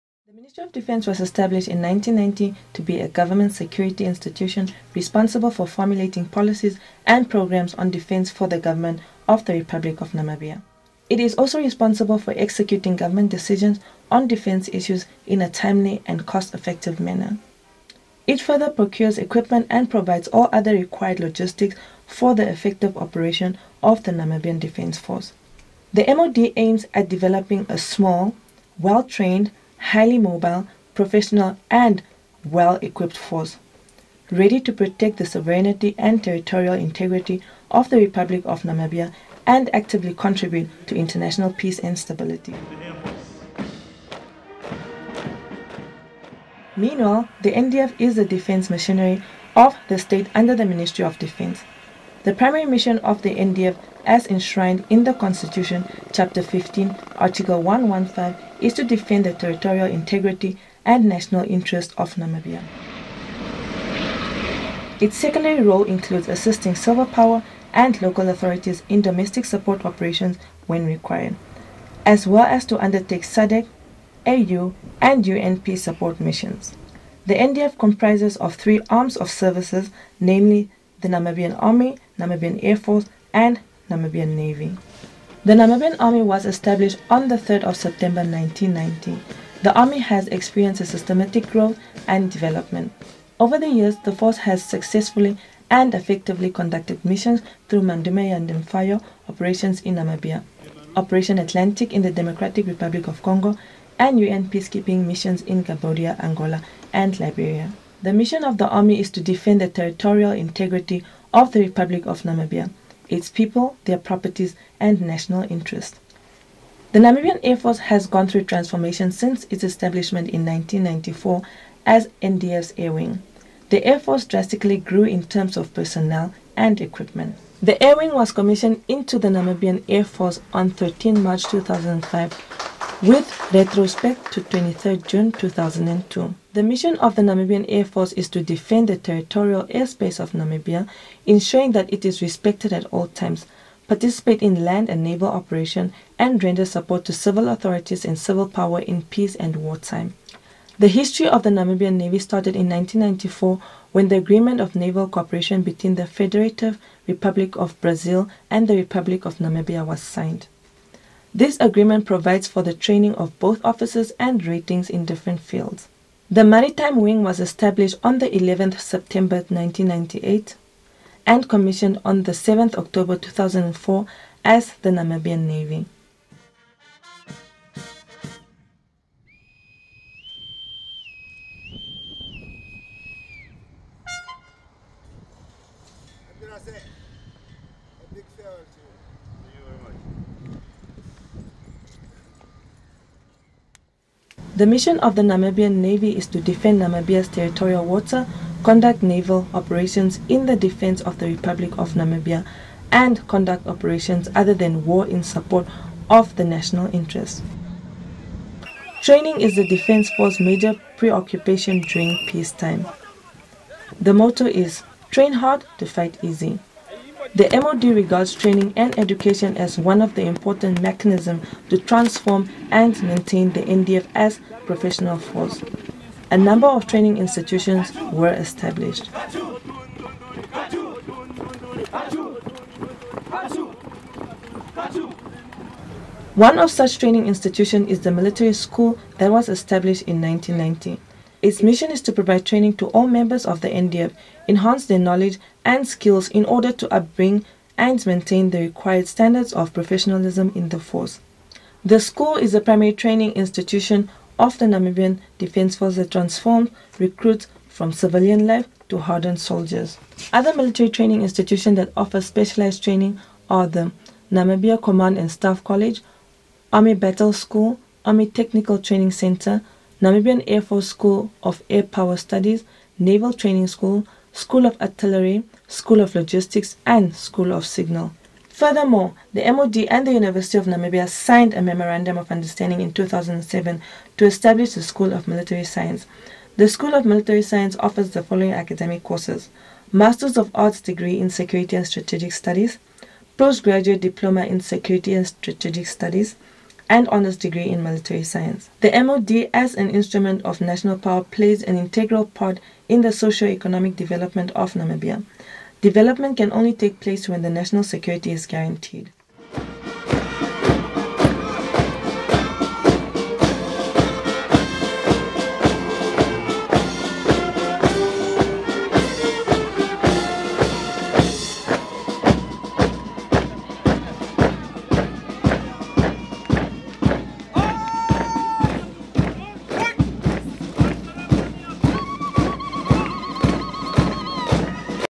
Namibian Ministry of Defence background
A brief background on the history and mandate of the Namibian Defence ministry